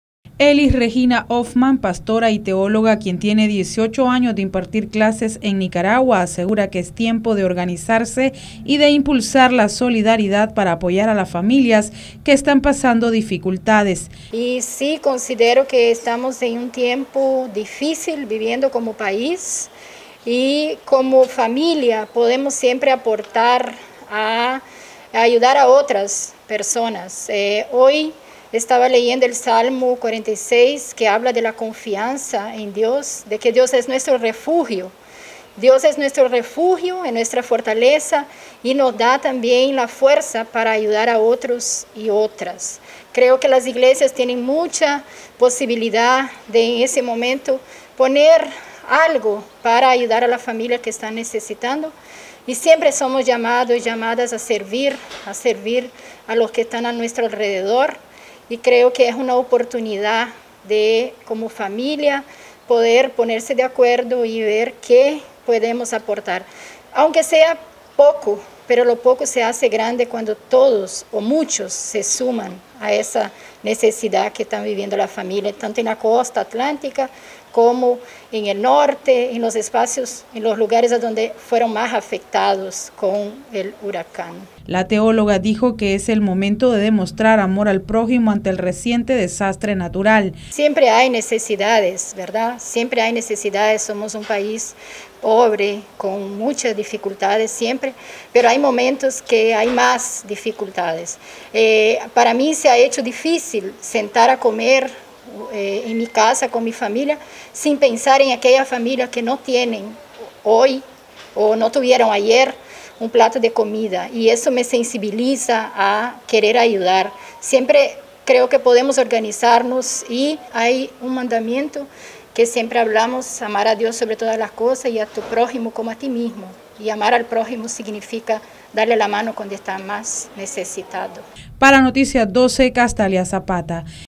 "Es momento de organizarse y demostrar amor al prójimo"
ETA NosResistimosAlEmbargo Es momento de organizarse y demostrar amor al prójimo, asegura pastora Elis Hoffmann.